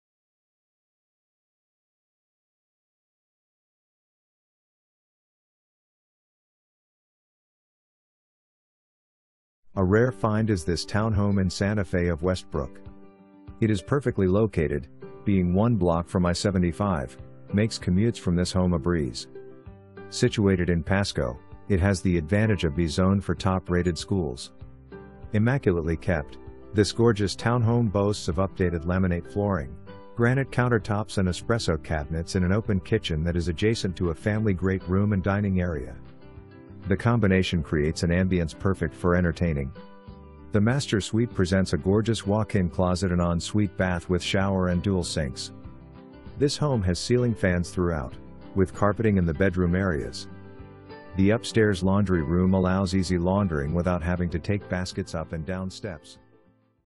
27540 DESERT WILLOW WAY
27540 DESERT WILLOW WAY
$218,000, 3 bed, 4.00 bath, 1,576 SF, MLS# U8113122

A rare find is this townhome in Santa Fe of Westbrook. It is perfectly located, being one block from I75, makes commutes from this home a breeze. Situated in Pasco, it has the advantage of be zoned for top rated schools. Immaculately kept, this gorgeous townhome boasts of updated laminate flooring, granite countertops and espresso cabinets in an open kitchen that is adjacent to a family great room and dining area. The combination creates an ambience perfect for entertaining. The master suite presents a gorgeous walk-in closet and en suite bath with shower and dual sinks. This home has ceiling fans throughout, with carpeting in the bedroom areas. The upstairs laundry room allows easy laundering without having to take baskets up and down steps. This home won't last long in this market.

2 full bath, 2 half bath

Sonja Costner
Align Right Realty